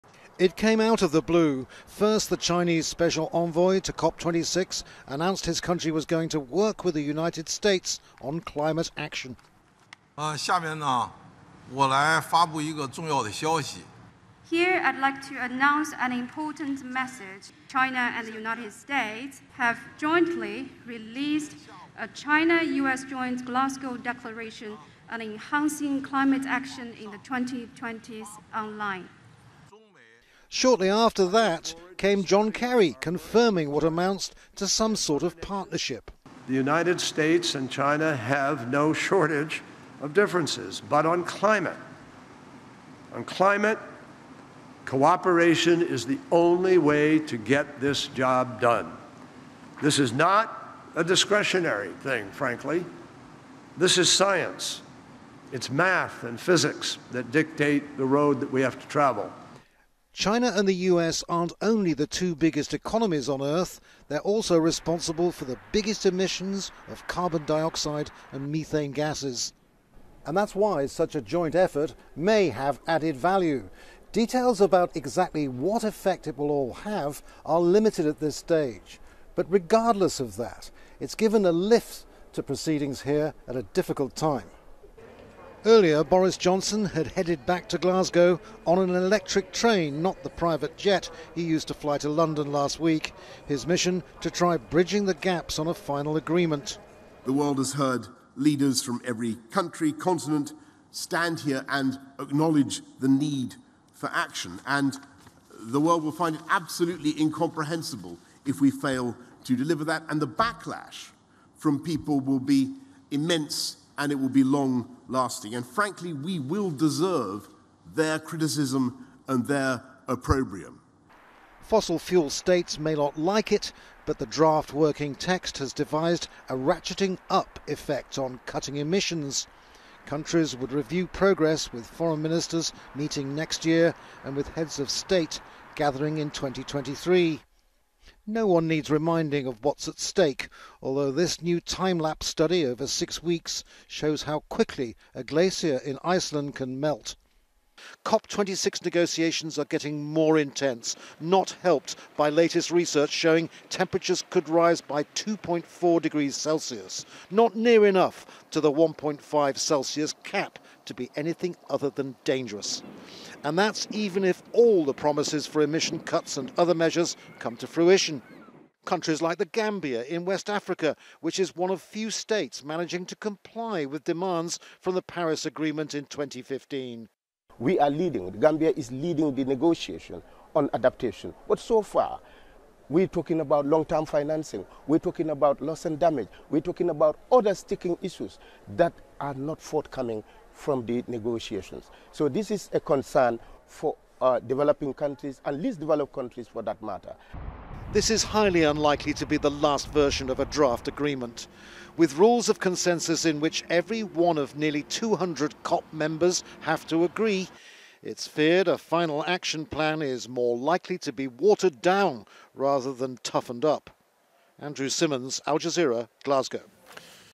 China and US announce deal to boost cooperation on climate change
China and the United States, the world’s two largest emitters of carbon dioxide, unveiled a deal to ramp up cooperation tackling climate change, including by reducing methane emissions, protecting forests and phasing out coal.
In a joint statement announced at the United Nations Climate Change Conference (COP26) in Glasgow, the countries said they had reached an agreement to redouble efforts to fight climate change with “concrete actions”.

Al Jazeera’s Andrew Simmons reports from Glasgow.